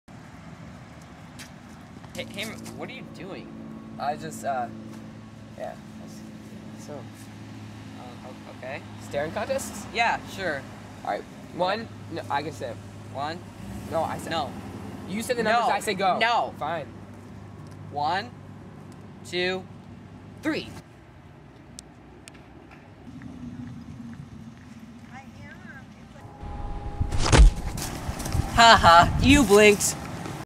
Fall Fun episode 5 Staring Contest!
to win a staring contest. simply do not blink